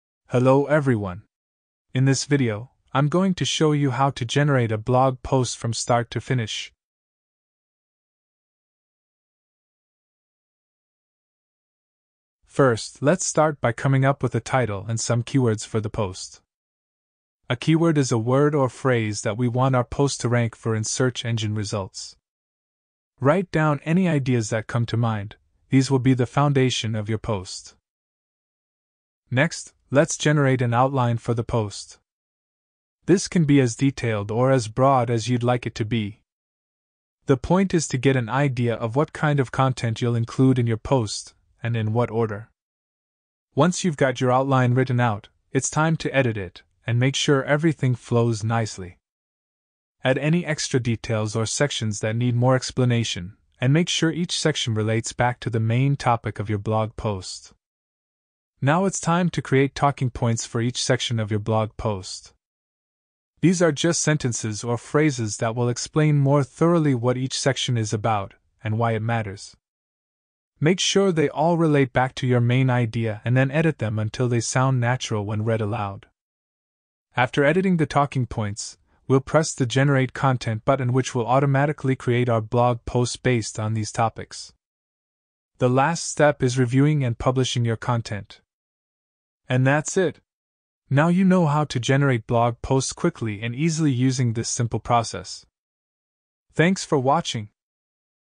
How to Create a 2500-Word Blog Post in 3 Minutes Using AI, from Start to Finish
Are you looking for a way to quickly create a 2500-word blog post without having to manually write it yourself? In this video, we'll show you how to use AI to create a 2500-word blog post in just 3 minutes, from start to finish! We'll show you how to quickly generate a post, edit it to make sure it reads naturally, and publish it online. Watch this video now to learn how to create a 2500-word blog post in no time!